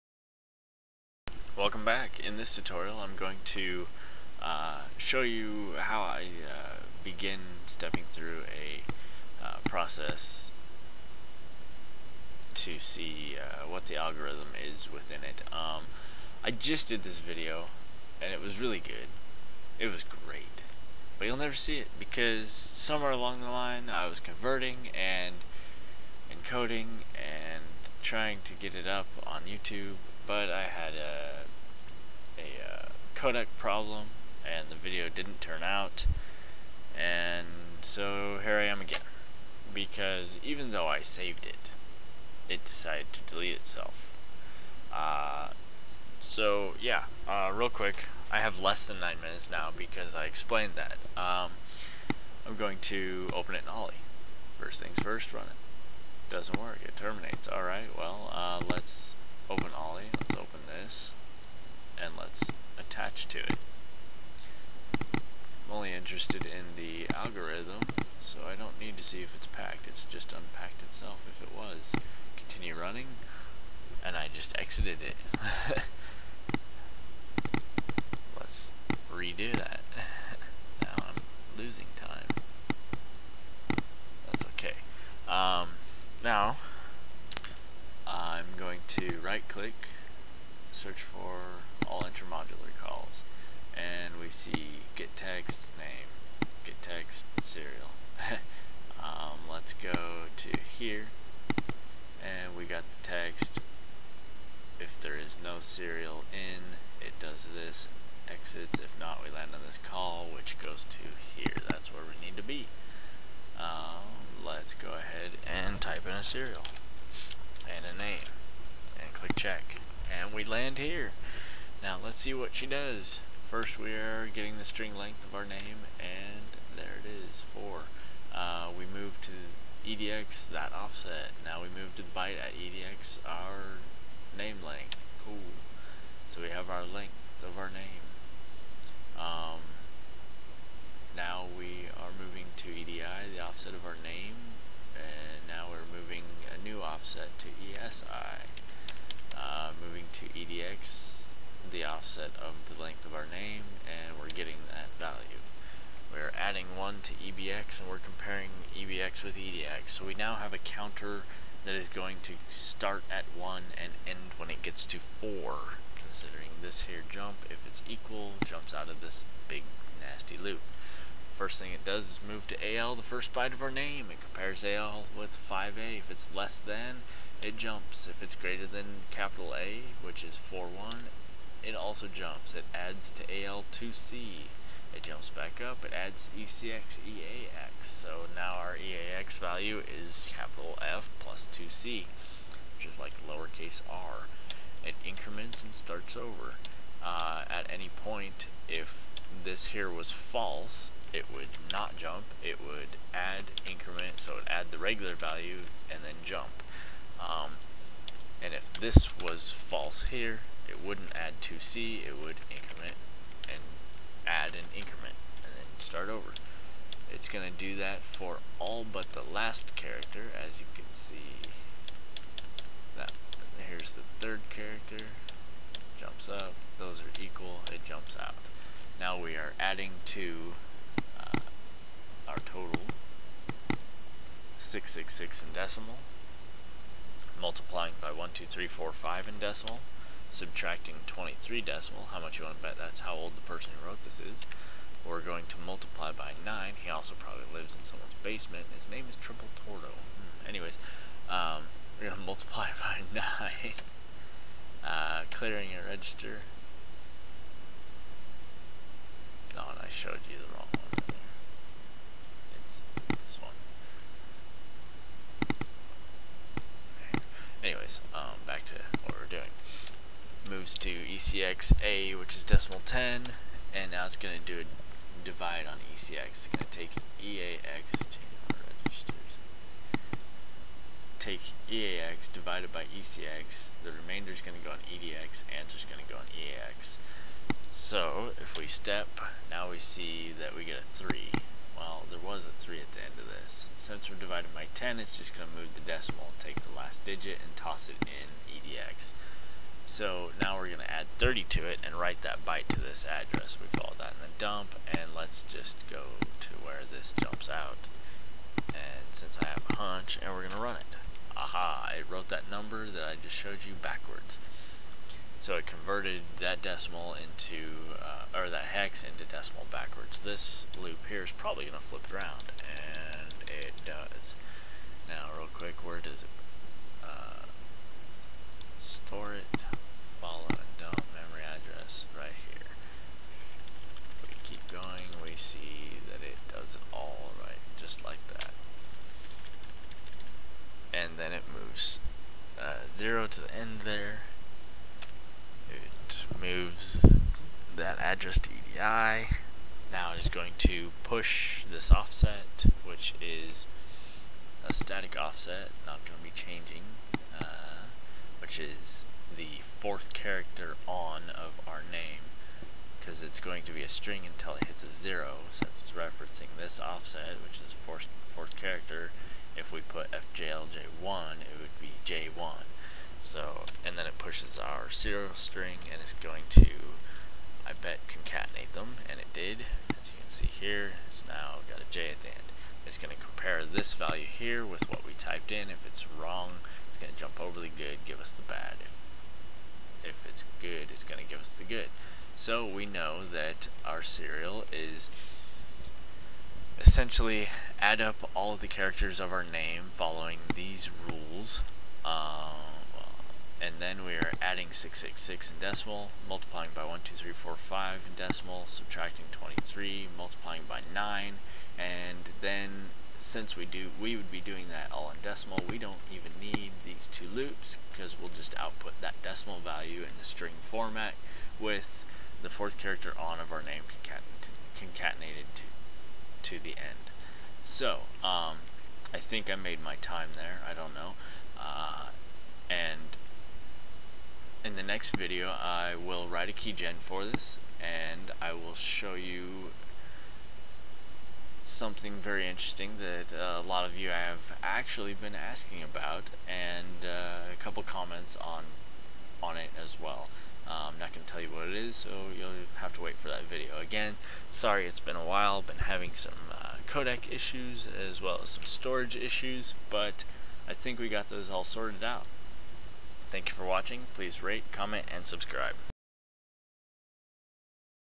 finding the algo 2
showing you how to find the algorithm of another program on 3rd look into the file... i would say first but I had seen it once in the past and i had to re-shoot this video as the original codec i was using seems to have gone nuts up on me and doesn't want to work anymore... but i am looking into this... I'm sorry for the quality changes of this video i was forced to change codecs and I'm not satisfied with the results... let me know what you think... is it ok or do i need to make sure and get back to the old one? the one plus to this one.. is that the end file size is about a fifth the size of the other videos.. but I'm not one to want to sacrifice quality just for size... i will tweak the settings and make this one look as good as i can for my next video until i can get my old one working again... but I'm rambling now... RATE COMMENT AND SUBSCRIBE!